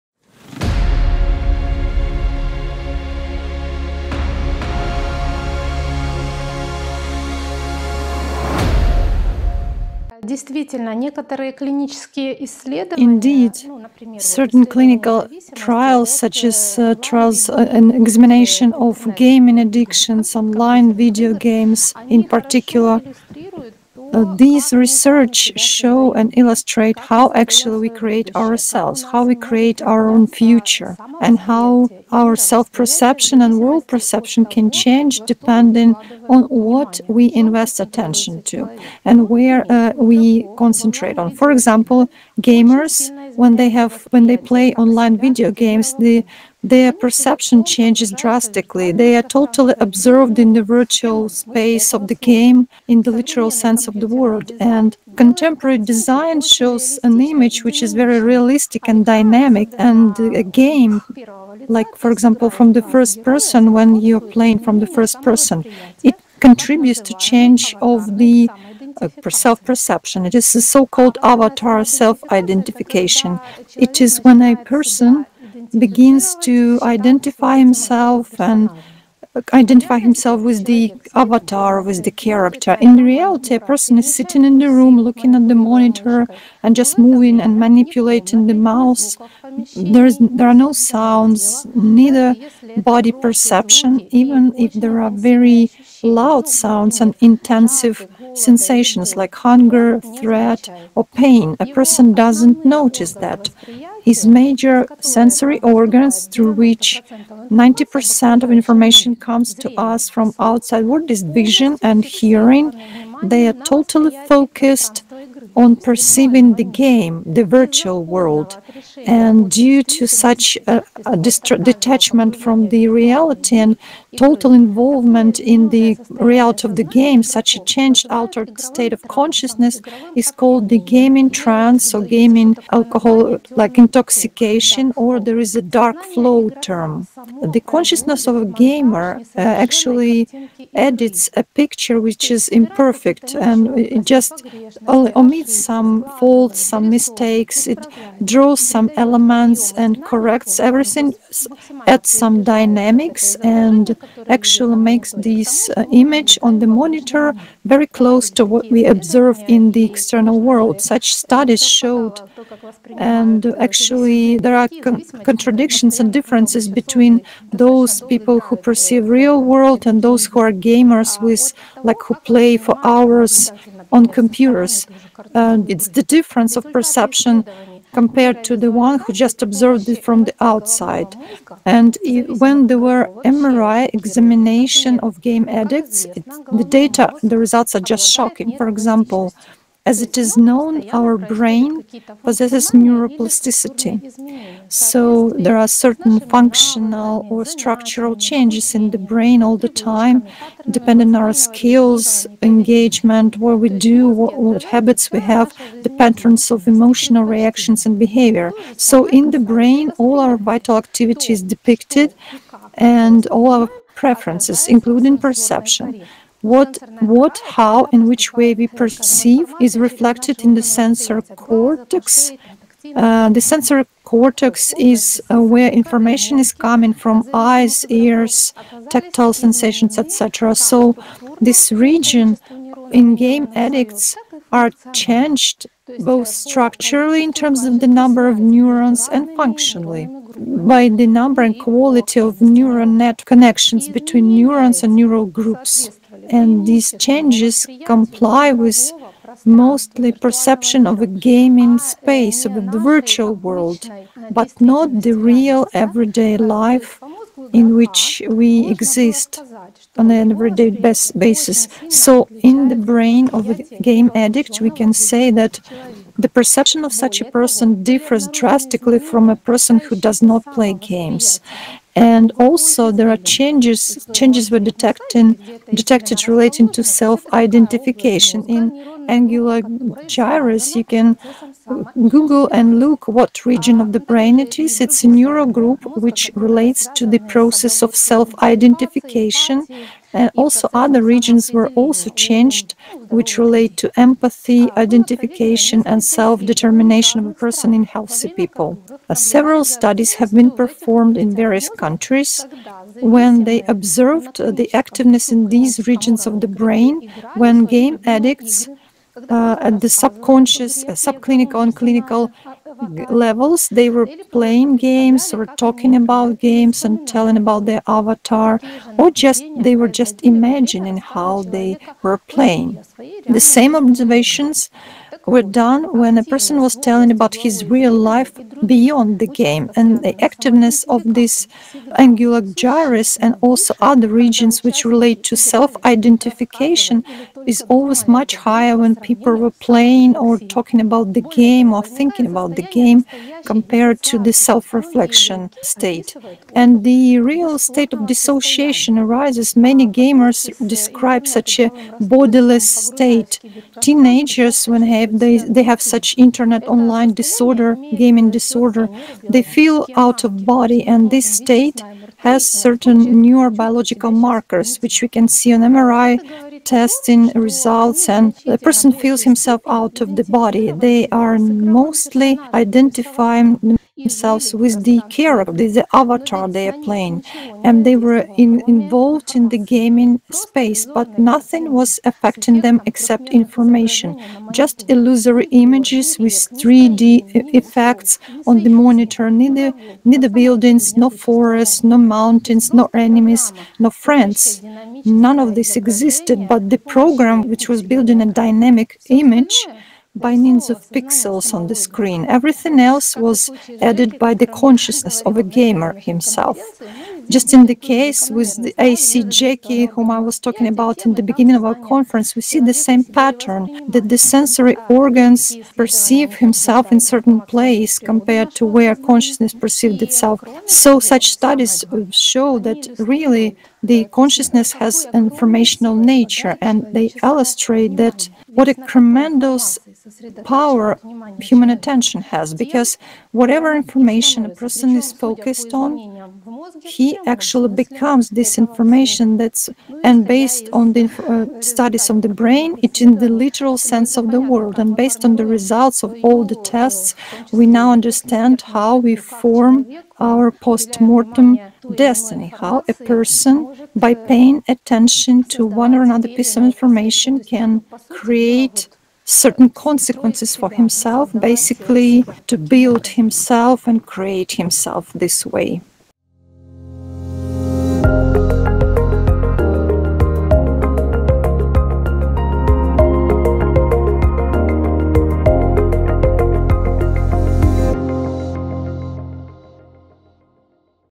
Gaming Addiction Takes Life
When a person is fully immersed in the space of the game, he considers himself its hero, meaning, an avatar, and his entire perception is focused on virtual reality. But does he live at this time? The game is just a set of pixels on the screen, a beautiful picture. And to this, the player gives all of his attention, the most valuable resource available to people.

A person who constantly plays virtual games has problems with self-identity, as confirmed by studies of the brain. Tatiana Zinchenko, Ph.D. in psychology, psychiatrist, rehabilitologist, and president of the International Association for the Study of Gaming Addictions (IASGA) from Switzerland tells about it at the International online conference "Life After Death. Fiction and Facts."